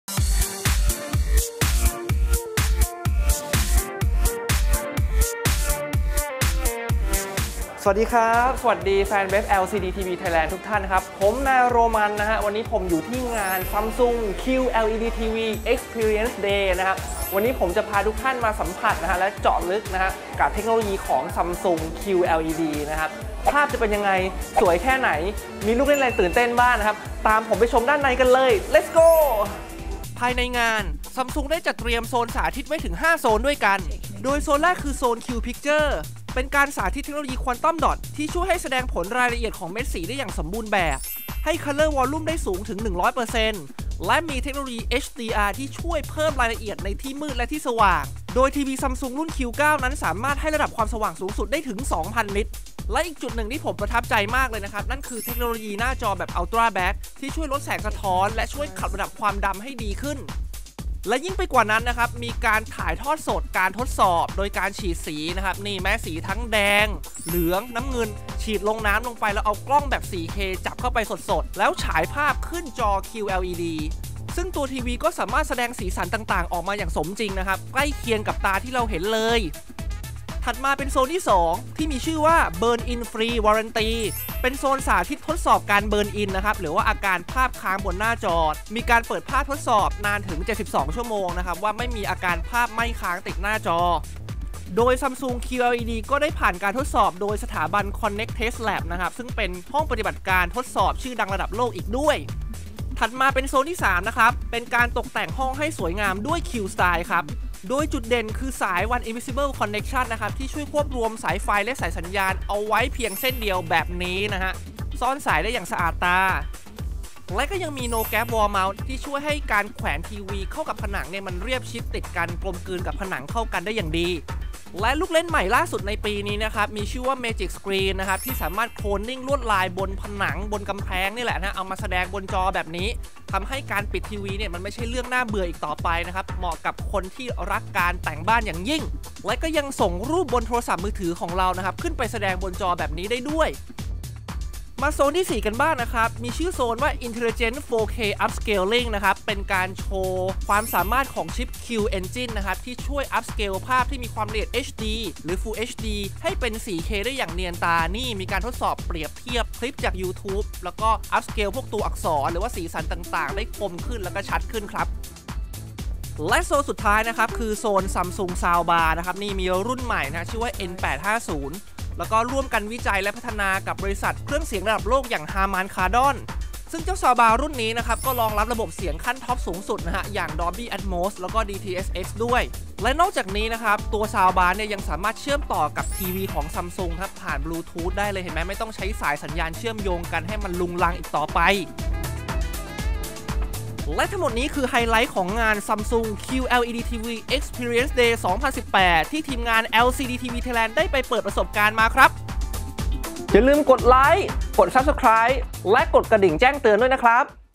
พาทัวร์งาน Samsung QLED Experience 2018 ร่วมเจาะลึกและสัมผัสกับนวัตกรรมทีวีอัฉริยะ !
ซัมซุง จัดกิจกรรม ‘QLED TV Experience Day 2018’ ร่วมเจาะลึกและสัมผัสกับนวัตกรรมทีวีอัฉริยะ Samsung QLED TV

ติดตามข่าวสาร รีวิว ทีวี เครื่องเสียง ได้ที่

เข้าร่วมกลุ่ม Facebook Group ต่างๆ ได้ที่